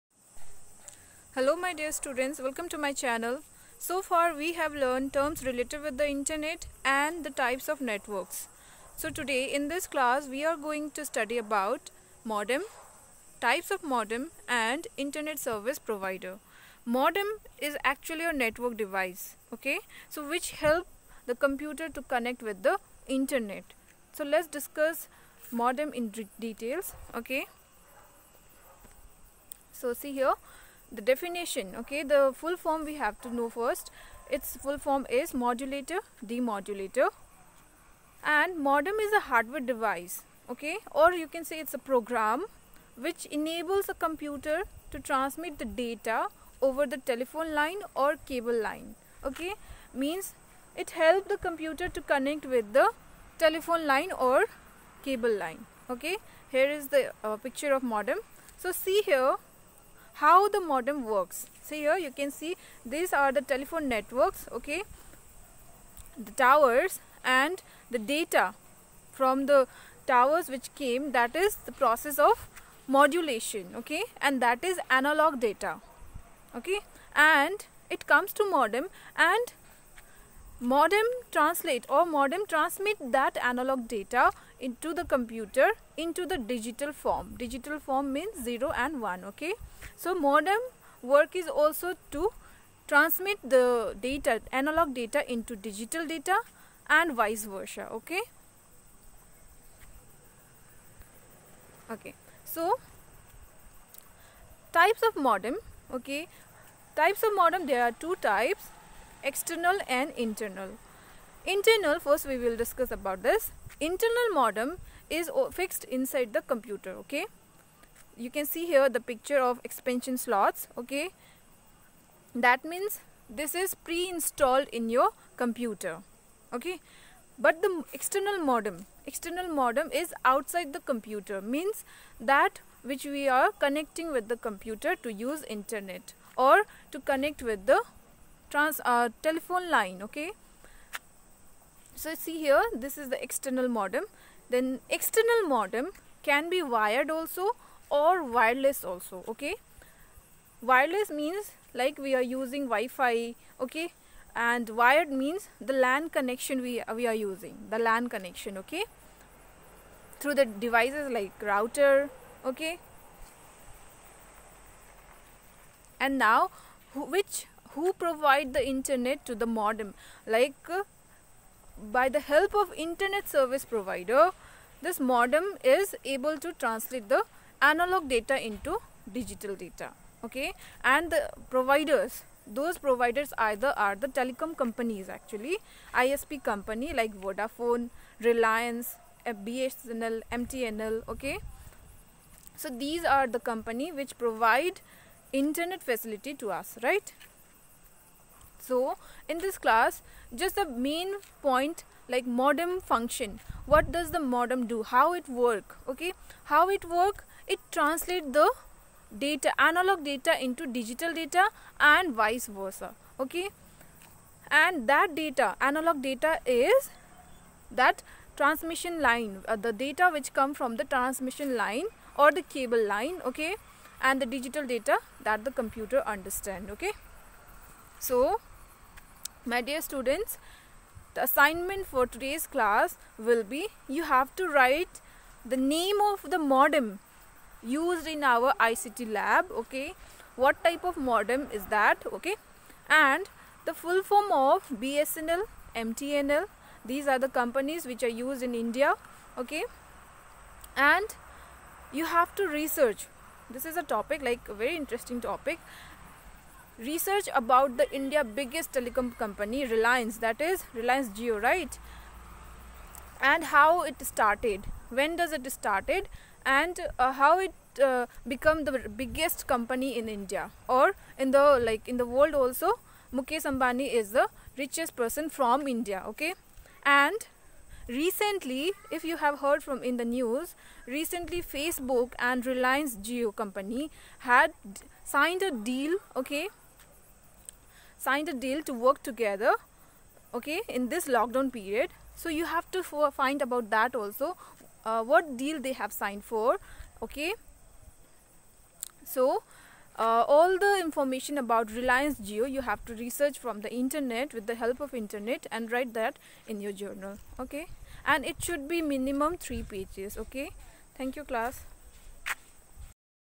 TYPES OF MODEM
this video explains modem definition and it's types and also ISP.